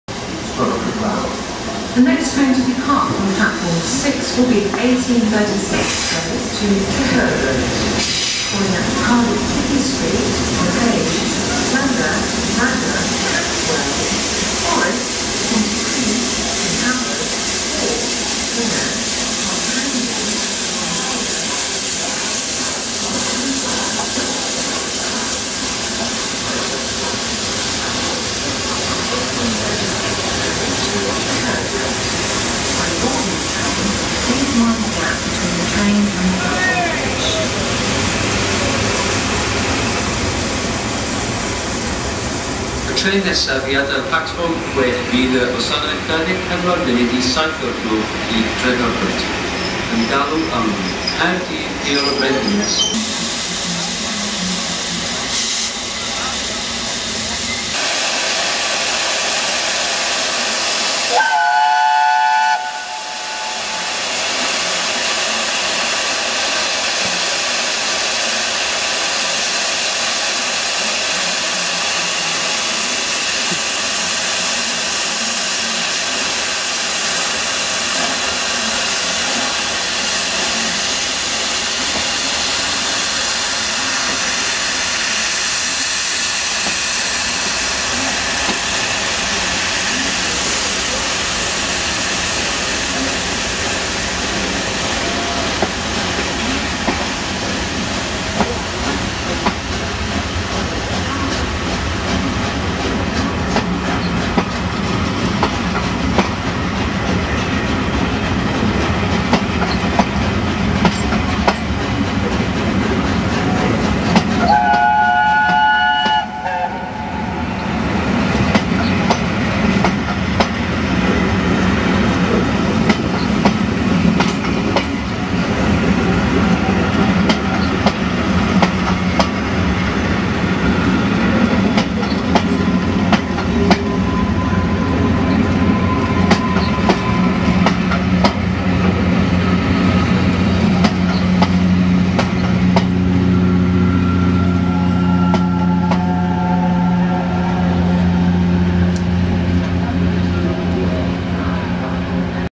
Tornado 60163 at Cardiff Central on St Davids Day
on a Charter to London.